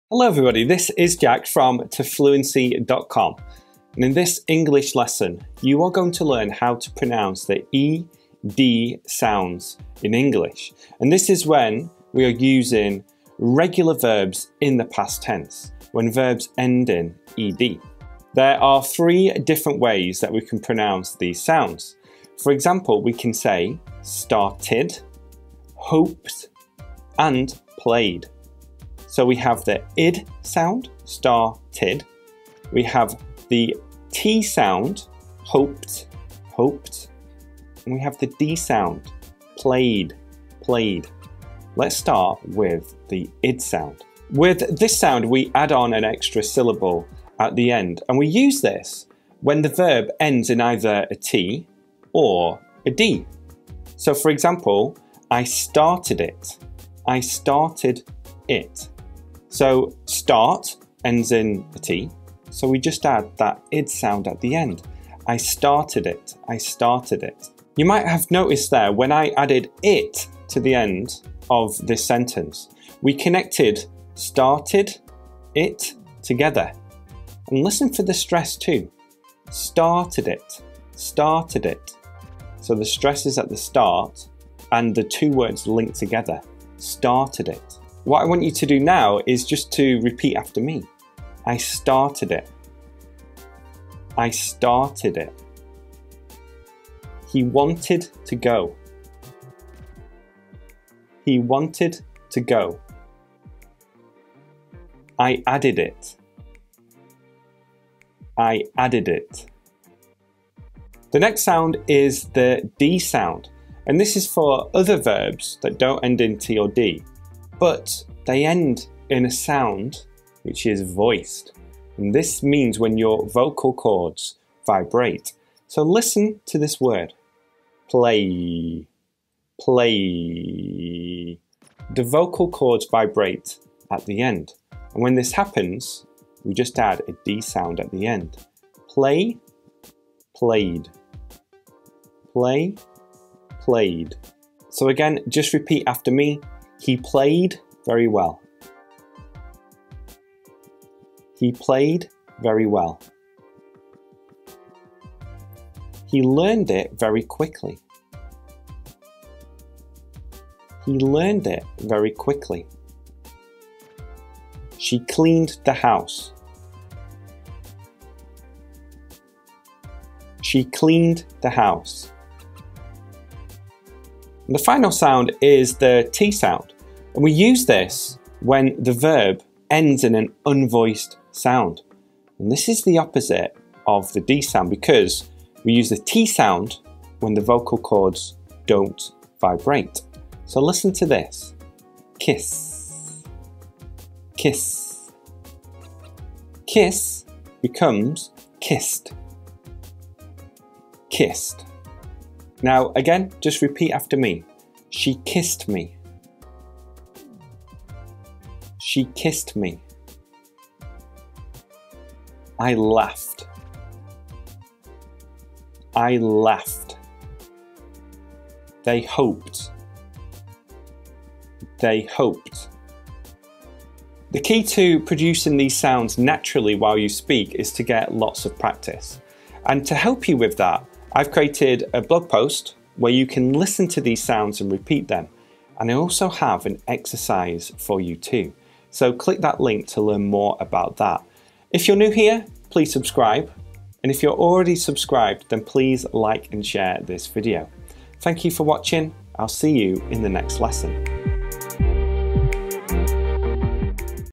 How to Pronounce ED at the End of Words in English | Regular Verbs in the Past
In this lesson, you're going to learn - and practice! - how to pronounce ED at the end of words in English.